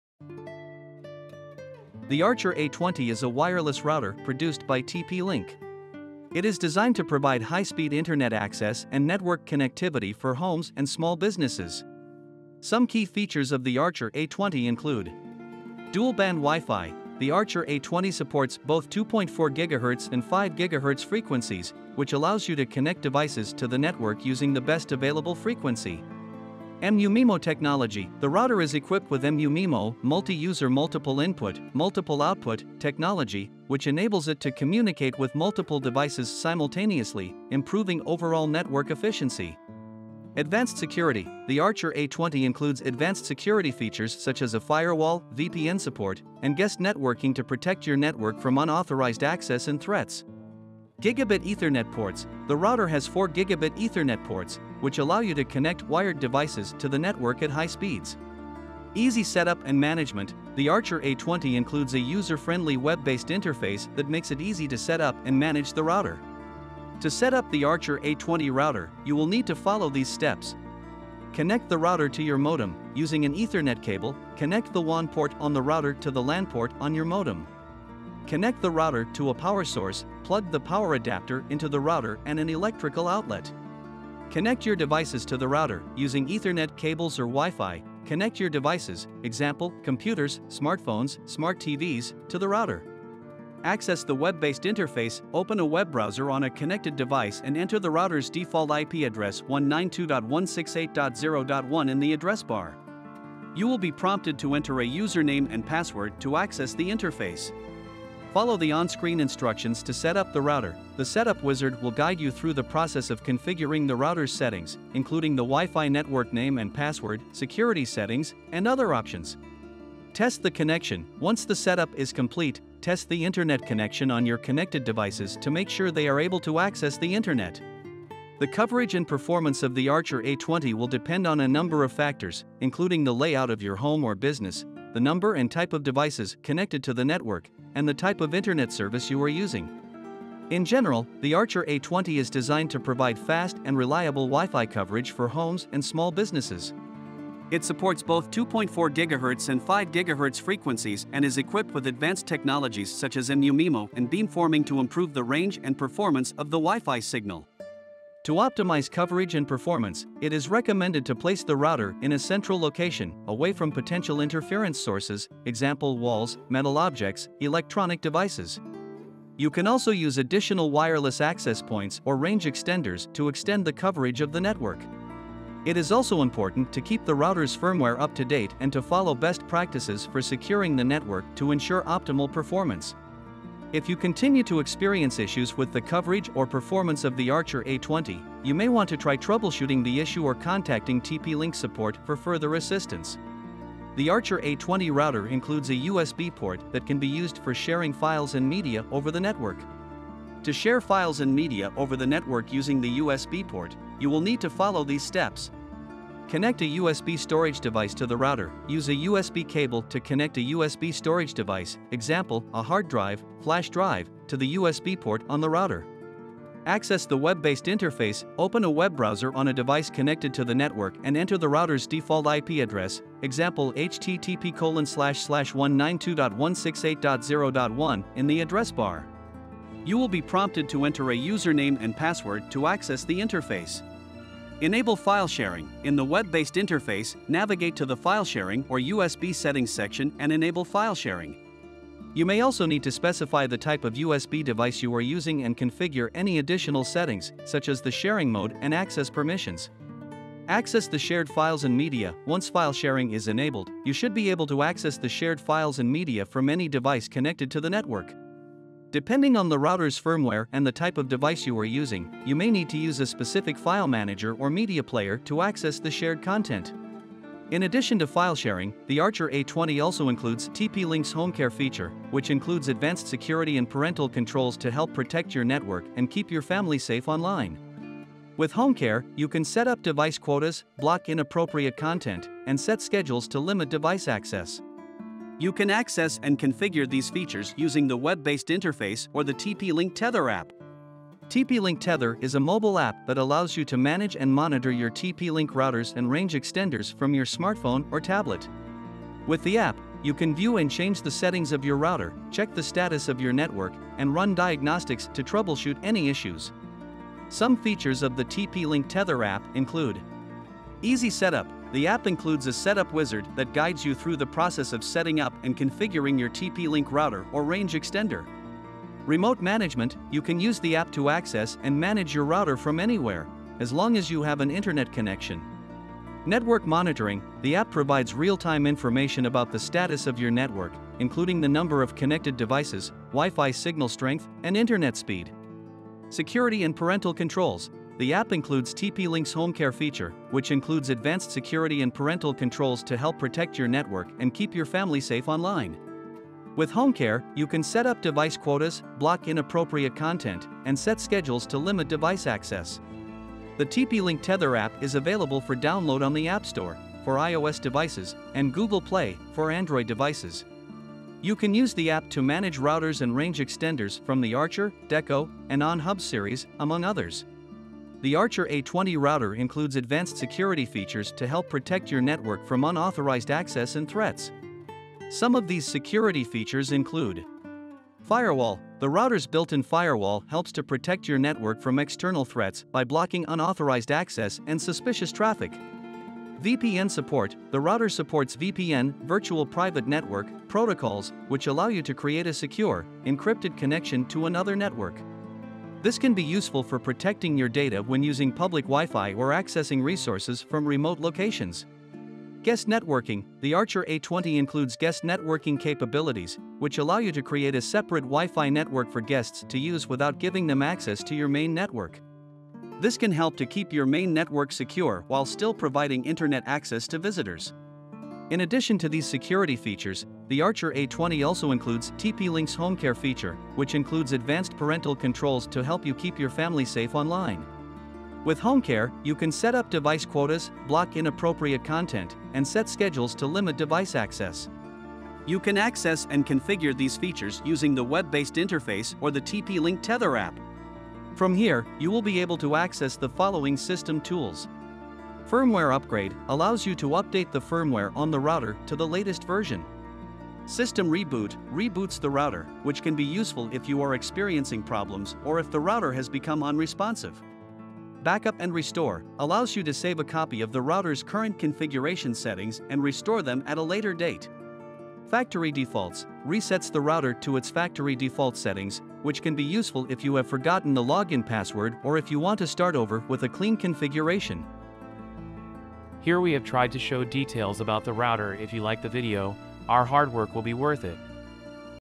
Setting up the Archer A20 Router | Archer A20 Tips and Tricks | AC4000 MU-MIMO Tri-Band Wi-Fi Router
The Archer A20 is a wireless router produced by TP-Link that is designed to provide high-speed Internet access and network connectivity for homes and small businesses. Some users have reported that the Archer A20 delivers fast and reliable Wi-Fi performance, with good coverage and a user-friendly web-based interface. Some key features of the Archer A20 include:

Dual-band Wi-Fi: The Archer A20 supports both 2.4 GHz and 5 GHz frequencies, which allows you to connect devices to the network using the best available frequency.

MU-MIMO technology: The router is equipped with MU-MIMO (Multi-User Multiple Input, Multiple Output) technology, which enables it to communicate with multiple devices simultaneously, improving overall network efficiency.

Advanced security: The Archer A20 includes advanced security features such as a firewall, VPN support, and guest networking to protect your network from unauthorized access and threats.

Gigabit Ethernet ports: The router has four Gigabit Ethernet ports, which allow you to connect wired devices to the network at high speeds.

Easy setup and management: The Archer A20 includes a user-friendly web-based interface that makes it easy to set up and manage the router.

Overall, the Archer A20 has received mostly positive reviews from users, with many praising its fast and reliable performance, advanced security features, and easy setup and management. However, some users have reported experiencing issues with the router's stability or range, and some have found the web-based interface to be somewhat confusing or difficult to navigate.